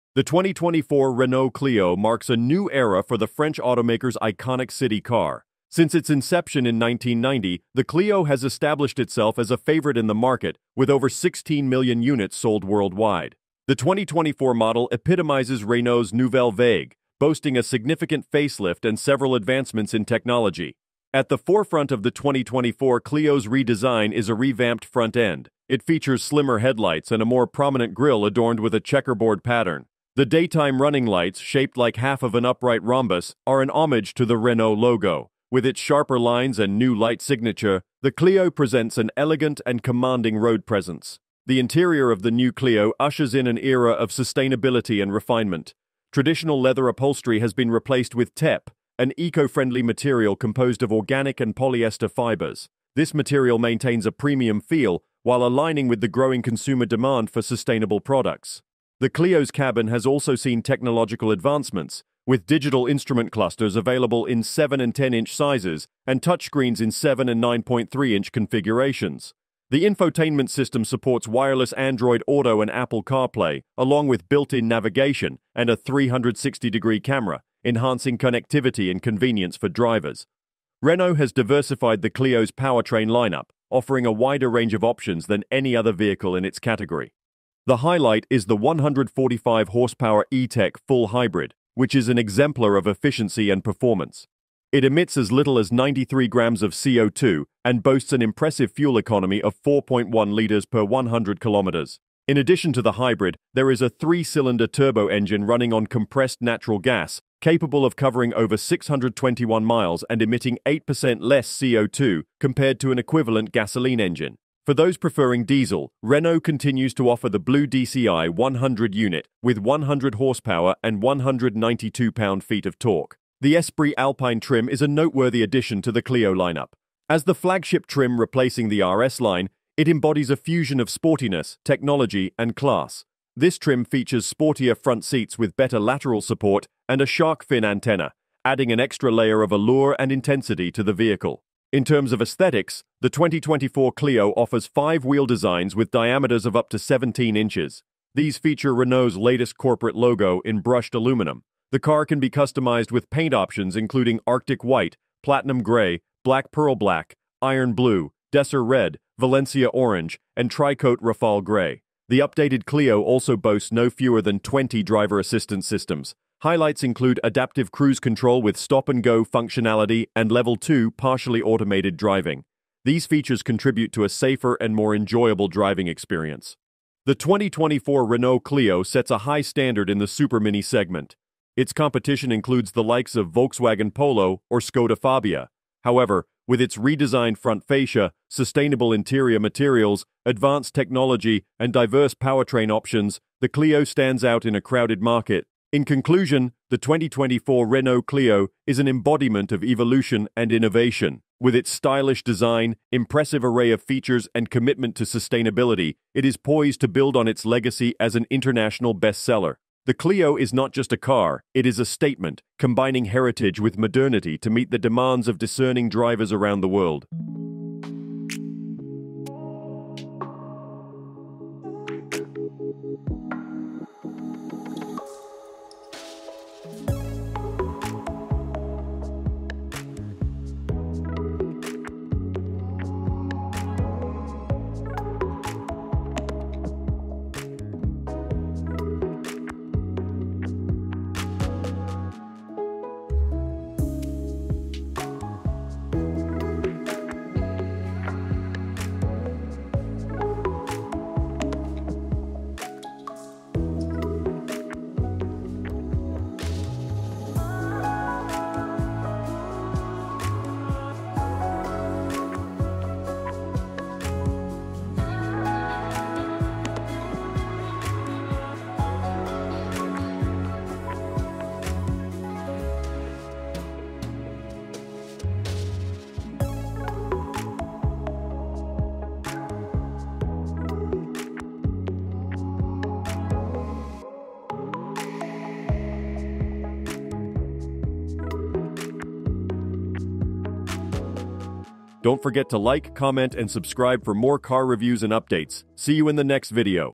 All NEW 2024 Renault Clio Esprit Alpine - Visual REVIEW interior, exterior, Driving
Renault has unveiled a significant mid-cycle update for its best-selling supermini, the Clio. The French automaker aims to keep the model fresh and competitive against segment rivals like the Volkswagen Polo, Skoda Fabia, and SEAT Ibiza. The fifth-generation Clio, launched just over four years ago, now sports a completely redesigned front featuring slimmer headlights and a wider grille with a checkerboard pattern.

As a nod to the Renault logo, the new Clio's daytime running lights are shaped like half of an upright rhombus. The company has also reintroduced clear taillights in the design. The Esprit Alpine, a flagship trim replacing the RS Line, showcases one of six available diffuser designs depending on the trim level and powertrain.

The 2024 Clio boasts a wider choice of powertrains than any other car in its category. The highlight is the 145 hp E-Tech full hybrid, which combines driving pleasure with savings, emitting as little as 93g CO2/km and achieving 4.1 L/100 km. Renault also offers a three-cylinder turbo engine running on compressed natural gas (CNG) and a diesel version in the form of the Blue dCi 100 unit.

The interior now features sustainable materials, replacing leather upholstery with TEP, a material made from organic and polyester fibers. The digital instrument cluster is available in 7- and 10-inch sizes, while the touchscreen comes in 7- and 9.3-inch configurations. The infotainment system supports wireless Android Auto and Apple CarPlay, built-in navigation, a 360-degree camera, and 20 driver assistance systems.

Renault offers the Clio with five wheel designs, all featuring the latest corporate logo in brushed aluminum. Paint options include Arctic White, Platinum Gray, Black Pearl Black, Iron Blue, Decir Red, Valencia Orange, and the new tri-coat Rafal Gray. The Clio's powertrain lineup starts with a naturally aspirated three-cylinder 1.0-liter engine, followed by a turbocharged 1.0-liter engine, and the flagship Clio E-Tech Full Hybrid 145.

The new Renault Clio aims to continue its legacy of success as a versatile, stylish, and efficient city car. With an updated design, advanced technology, and a wide range of powertrain options, the Clio is poised to maintain its position as a category leader in Europe and a favorite among customers around the world.